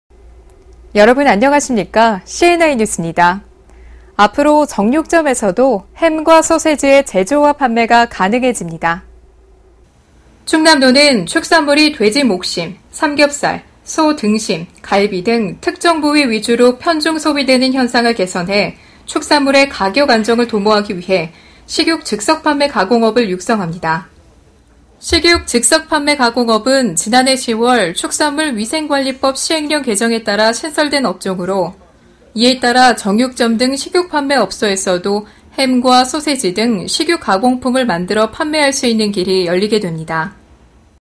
정육점서 햄·소시지 제조·판매 가능해진다
-'식육즉석판매가공업' 시설개선 자금·원료구입비 등 지원
-지난해 10월 축산물 위생관리법 시행령개정에 따라 신설

(리드멘트)
여러분 안녕하십니까? CNi뉴스입니다.
앞으로 정육점에서도
햄과 소시지의 제조와 판매가 가능해집니다.

충남도는
축산물이 돼지 목심·삼겹살, 소 등심·갈비 등
특정부위 위주로 편중 소비되는 현상을 개선해
축산물의 가격안정을 도모하기 위해
식육즉석판매가공업을 육성합니다.

식육즉석판매가공업은 지난해 10월
축산물 위생관리법 시행령개정에 따라 신설된 업종으로,
이에 따라 정육점 등 식육판매 업소에서도
햄과 소시지 등
식육가공품을 만들어 판매할 수 있는 길이 열리게 됩니다.